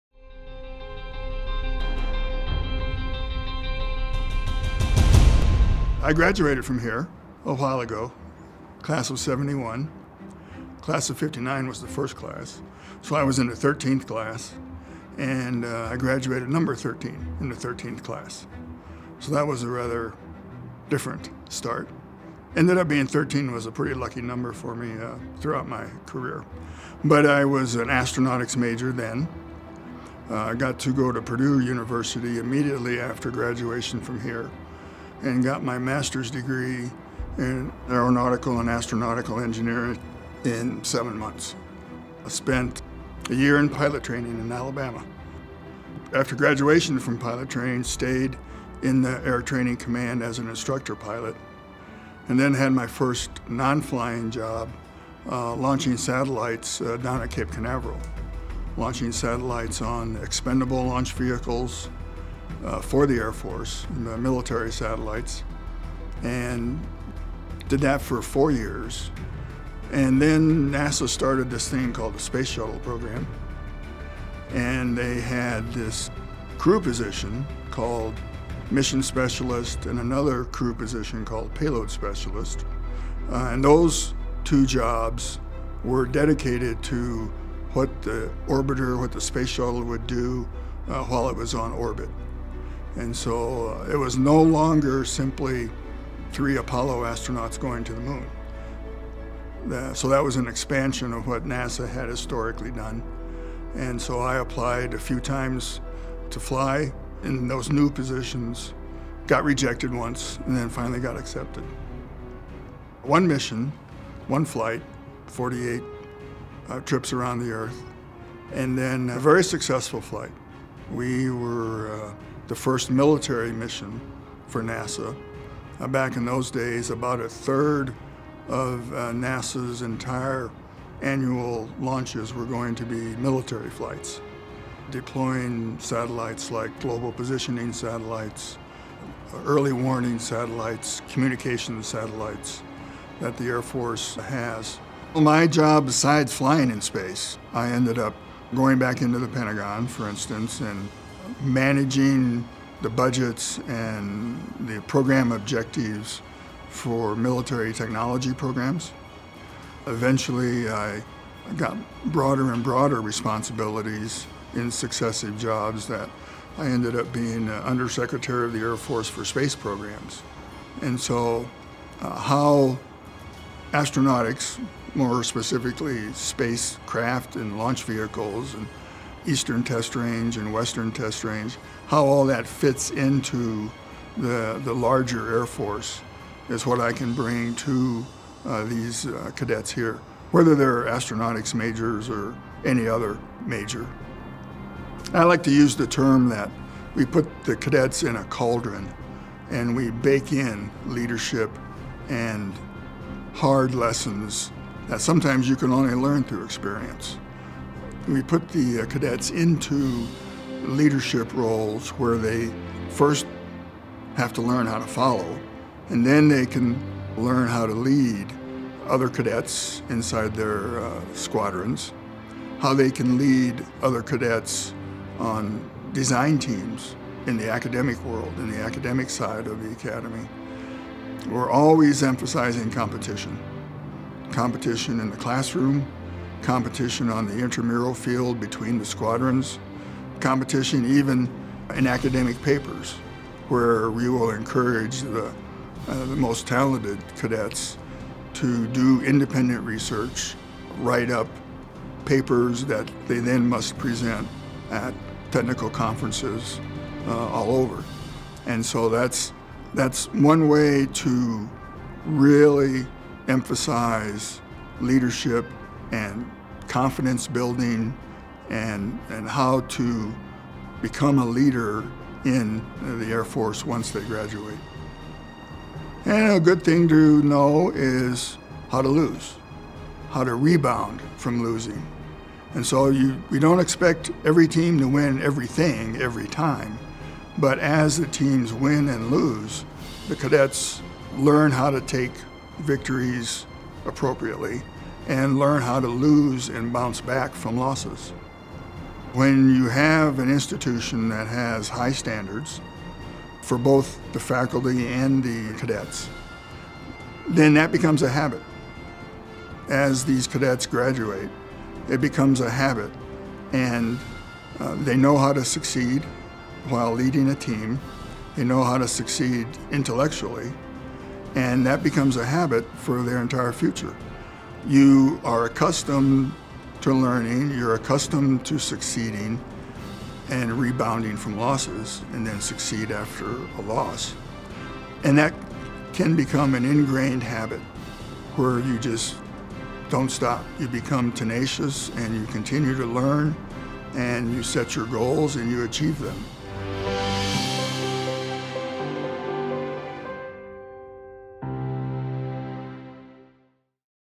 ARDI Professor: Gary Payton Full Length Video Profile
Full length video profile featuring ARDI distinguished professor, Gary Payton